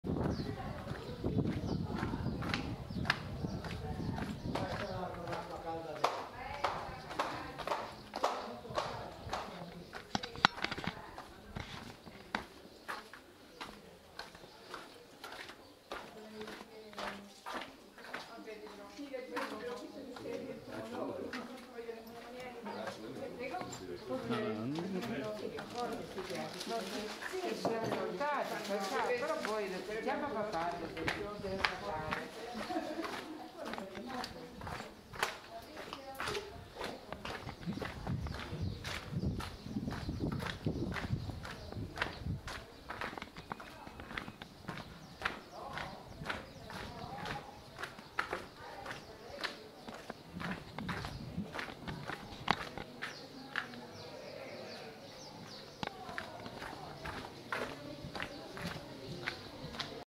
Mercato della Badia a Bivongi - Preparazione per la sera a 360° - Parte 2
Un giro tra i vicoli di Bivongi nel tardo pomeriggio  in preparazione per il mercato della Badia serale

Parte 2